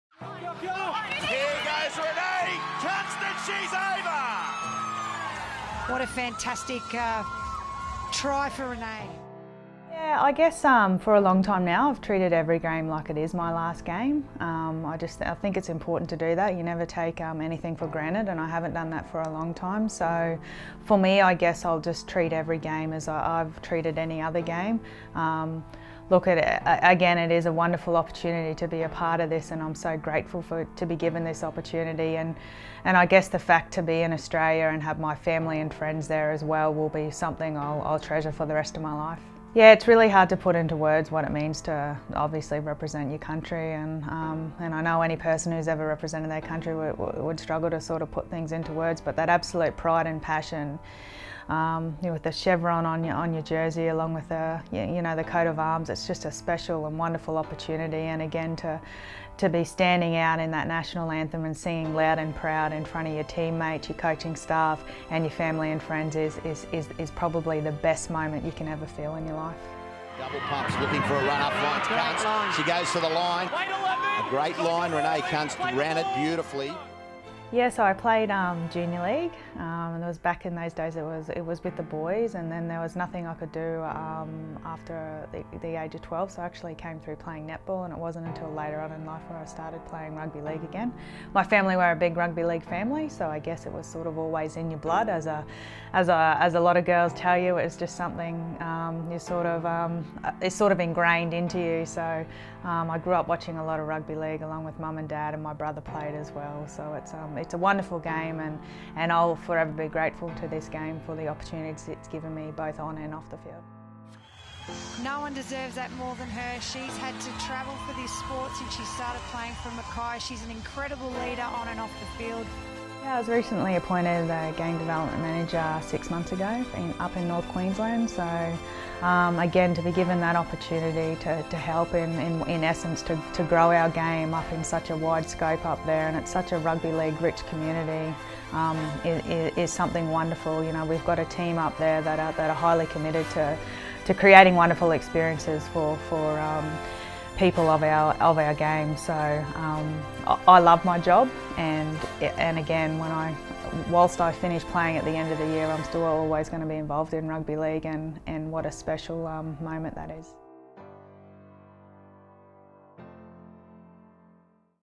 Renae Kunst - Jillaroos Interview
Video made for the Jilaroos page

Retiring Jilaroos co-captain Renae Kunst sits down to speak about what Rugby League means to her.